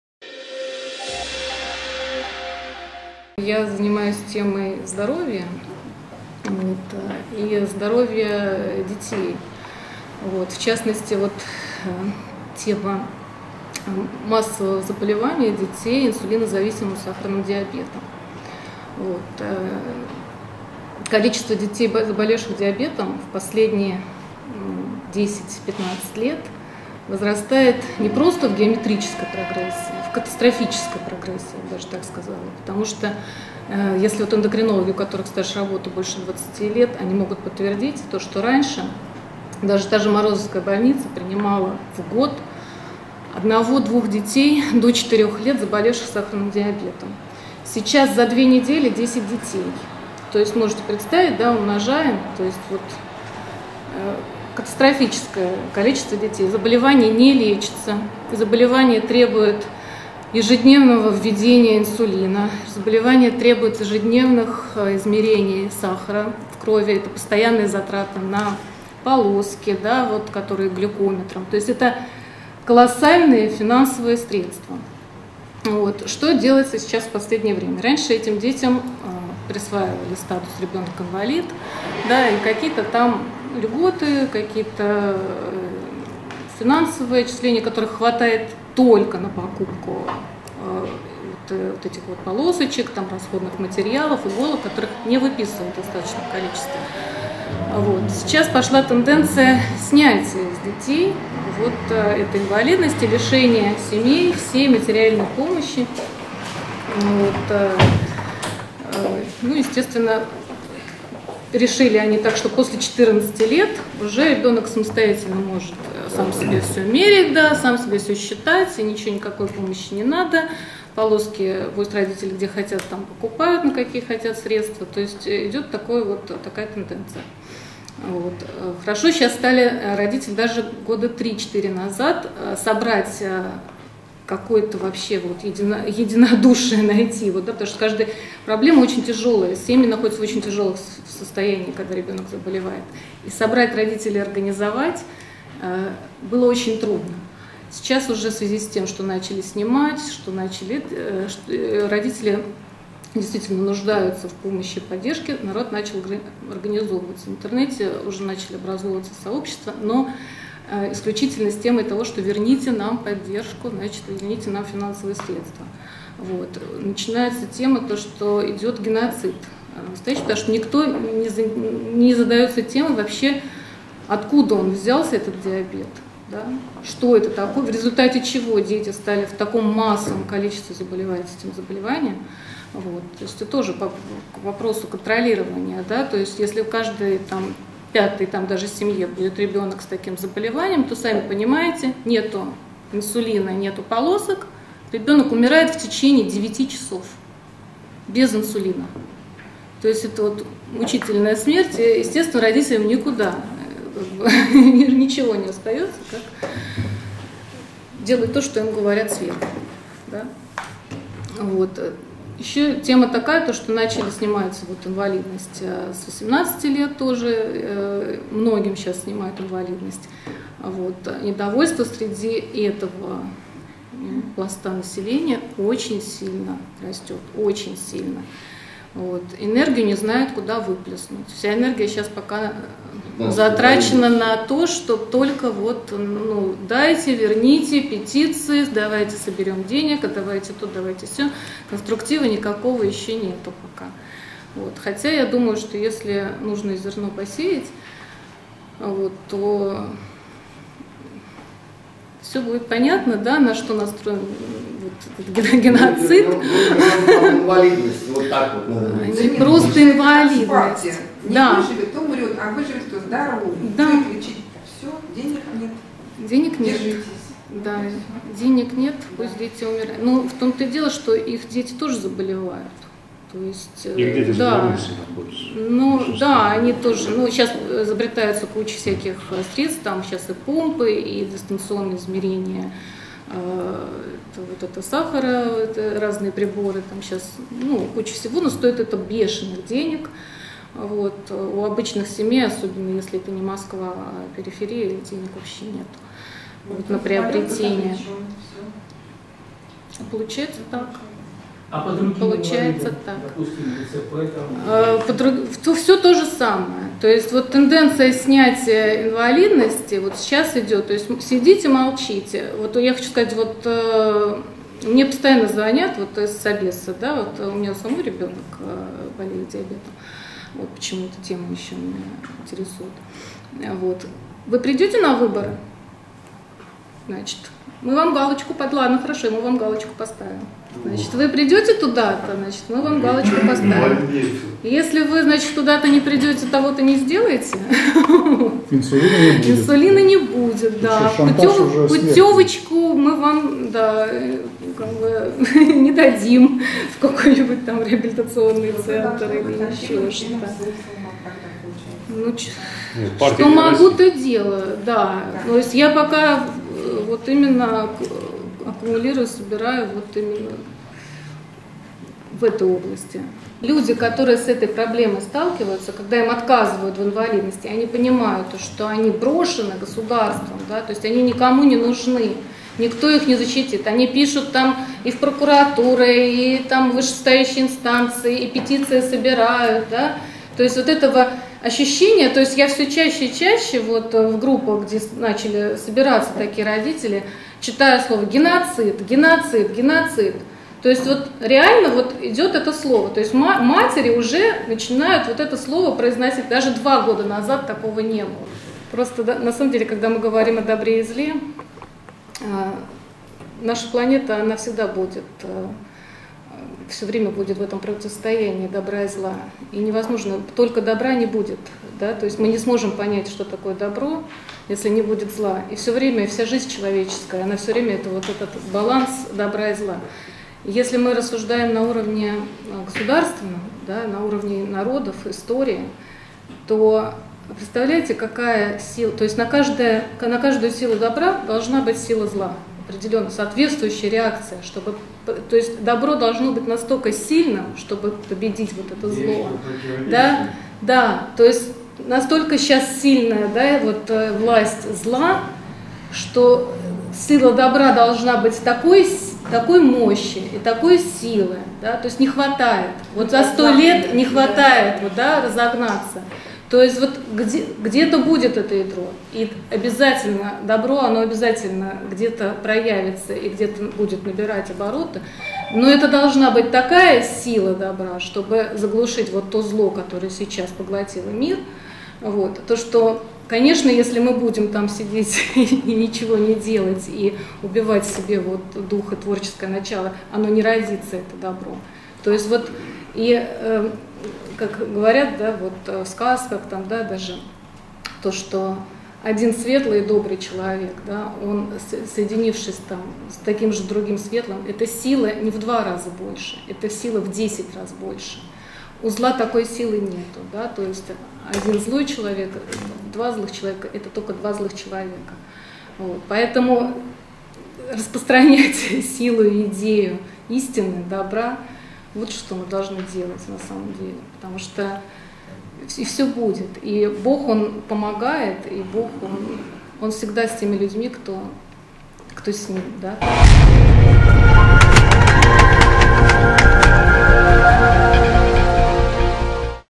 ВР-Клуб. Мы все чаще говорим о геноциде
Психолог Анжелика Лацис говорит о резком росте смертельно опасных заболеваний и полном равнодушии чиновников в больным детям. Родители все чаще стали называть деятельность чиновников  "геноцидом".

Поддержка проекта:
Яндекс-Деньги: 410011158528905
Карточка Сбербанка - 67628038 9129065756